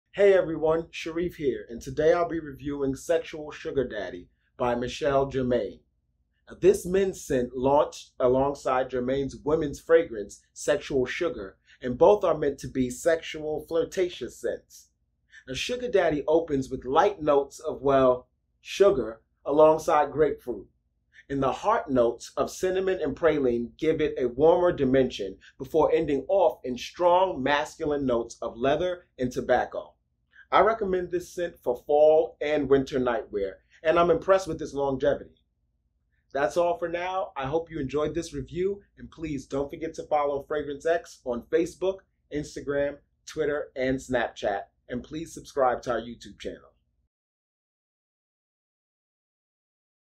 Sexual Sugar Daddy Cologne
Shariff reviews Sexual Sugar Daddy by Michel Germain.

TRANSCRIPT:
Hey everyone, Shariff here, and today I'll be reviewing Sexual Sugar Daddy by Michel Germain.  This men's scent launched alongside Germain's women's fragrance, Sexual Sugar, and both are meant to be sexy, flirtatious scents.

Sugar daddy opens with light notes of, well, sugar, alongside grapefruit.  In the heart, notes of cinnamon and praline give it a warmer dimension, before ending off in strong, masculine notes of leather and tobacco.  I recommend this scent for fall and winter night wear, and I'm impressed with its longevity.